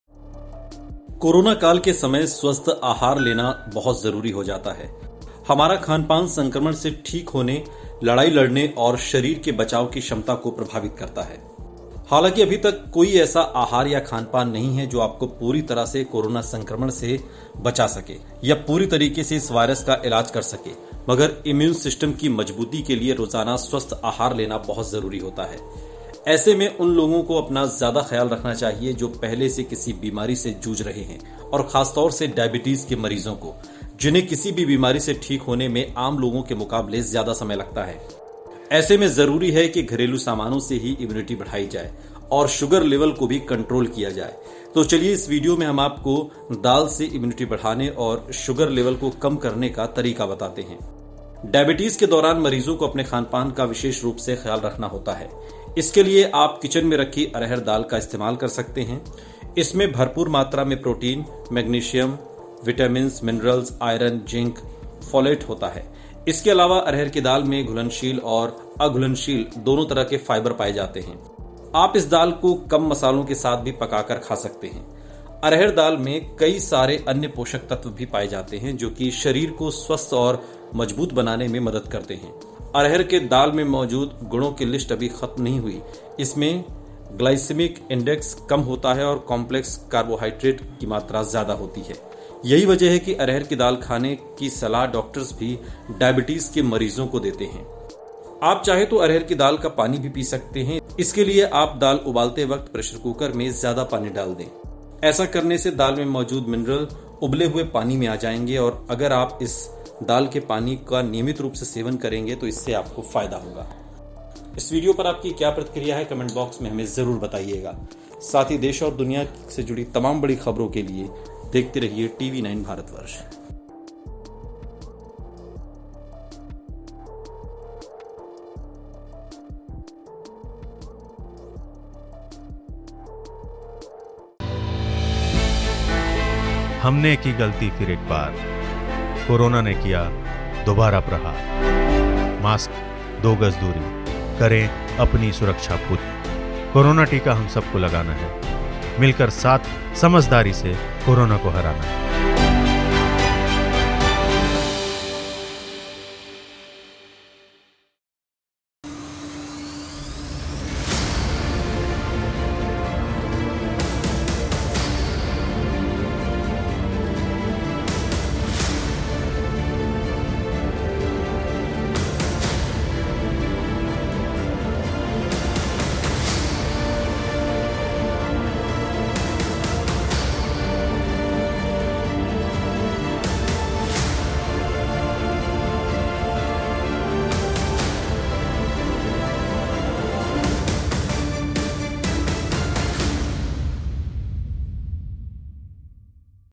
Diabetes Patients के लिए जरूरी है Arhar Dal का सेवन, Immunity बढ़ाने के साथ Sugar Level रहेगा कम
Corona Pandemic के समय Healthy Diet लेना बहुत जरूरी हो जाता है. हमारा खान-पान संक्रमण से ठीक होने, लड़ाई लड़ने और शरीर के बचाव की क्षमता को प्रभावित करता है. हालांकि अभी तक कोई ऐसा आहार या खान-पान नहीं है जो आपको पूरी तरह से कोरोना संक्रमण से रोक या इलाज कर सके.

Credits: Health | Priyanksha/Producer | MansiTyagi/Editor